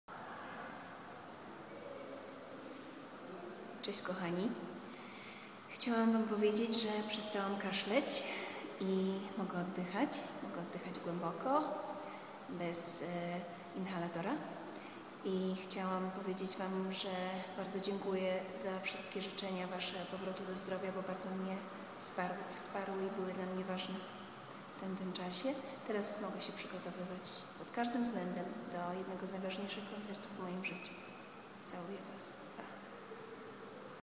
Edyta Górniak: wracam do zdrowia!
Edyta Górniak wraca do zdrowia i już nie może się doczekać najważniejszego koncertu w jej życiu. 22 kwietnia w Ergo Arenie na granicy Gdańska i Sopotu wystąpi obok José Carrerasa.